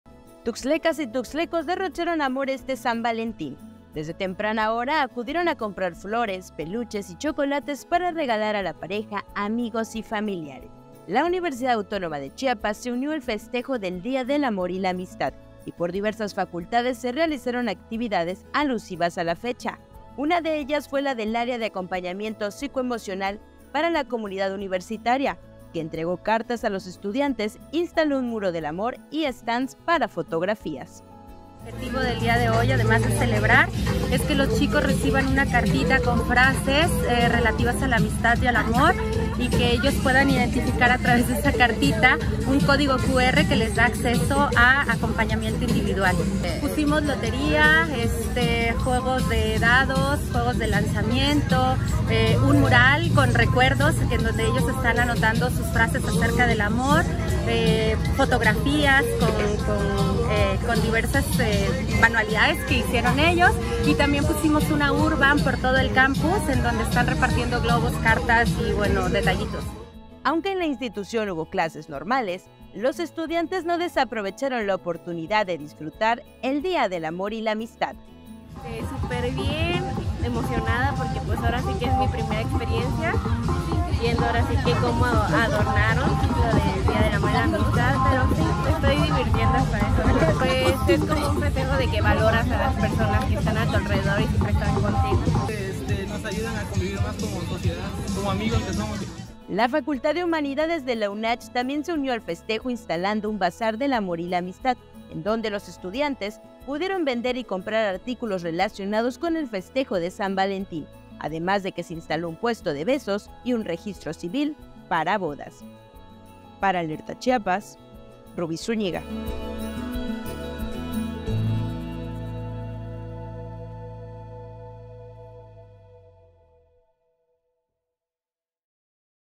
Así celebraron en Tuxtla el Día de San Valentín
Desde temprano realizaron compras de flores, peluches y chocolates para dar regalos.